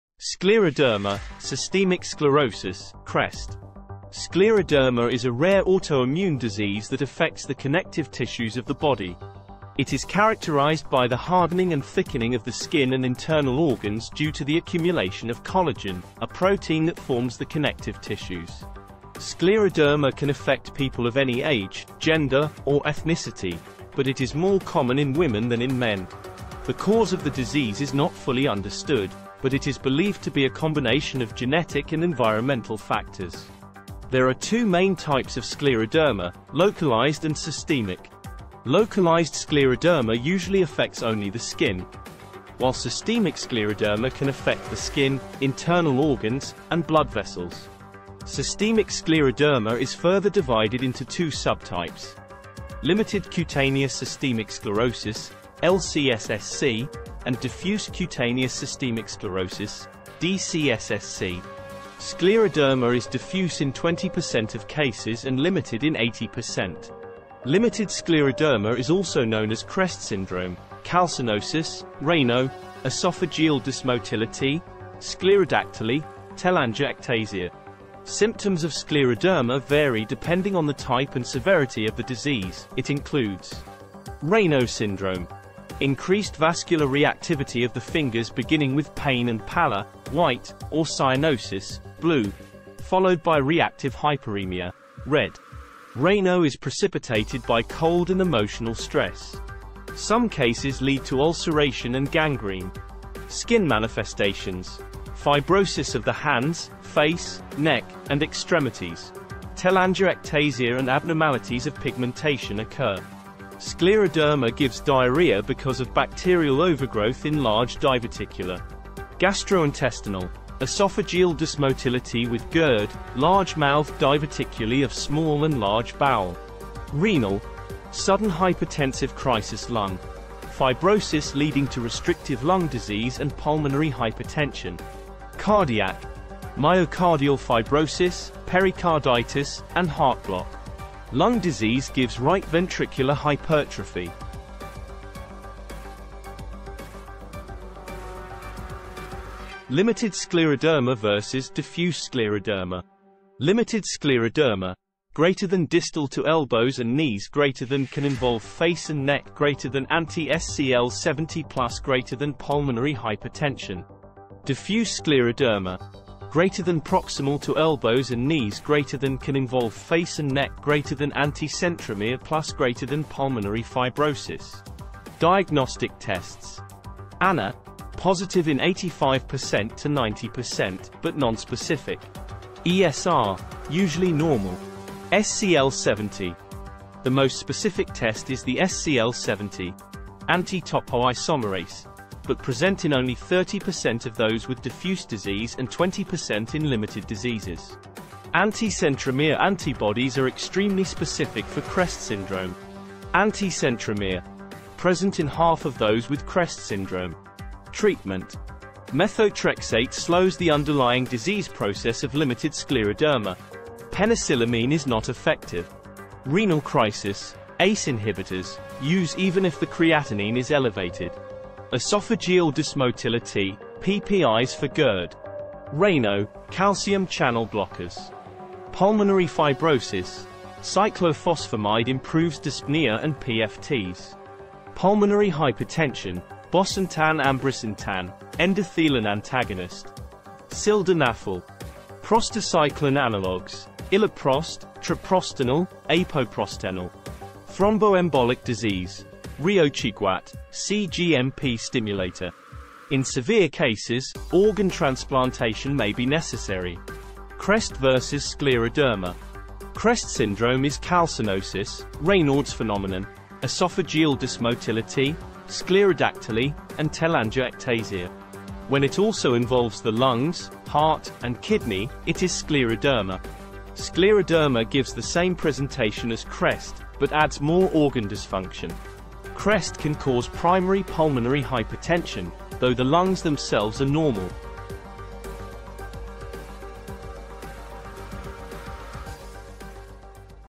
Scleroderma (Systemic Sclerosis)| CREST
This video is about scleroderma. Scleroderma is a rare autoimmune disease that affects the connective tissues of the body. It is characterized by the hardening and thickening of the skin and internal organs due to the accumulation of collagen, a protein that forms the connective tissues.

Scleroderma can affect people of any age, gender, or ethnicity, but it is more common in women than in men. The cause of the disease is not fully understood, but it is believed to be a combination of genetic and environmental factors.

There are two main types of scleroderma: localized and systemic. Localized scleroderma usually affects only the skin, while systemic scleroderma can affect the skin, internal organs, and blood vessels. Systemic scleroderma is further divided into two subtypes: limited cutaneous systemic sclerosis (lcSSc) and diffuse cutaneous systemic sclerosis (dcSSc).

Symptoms of scleroderma vary depending on the type and severity of the disease but can include skin thickening, Raynaud's phenomenon (a condition in which the fingers and toes become numb and cold in response to cold temperatures or stress), joint pain, fatigue, and shortness of breath.

Treatment for scleroderma is aimed at managing symptoms and preventing complications. This may include medications to control inflammation, immunosuppressive drugs, and physical therapy. In severe cases, organ transplantation may be necessary.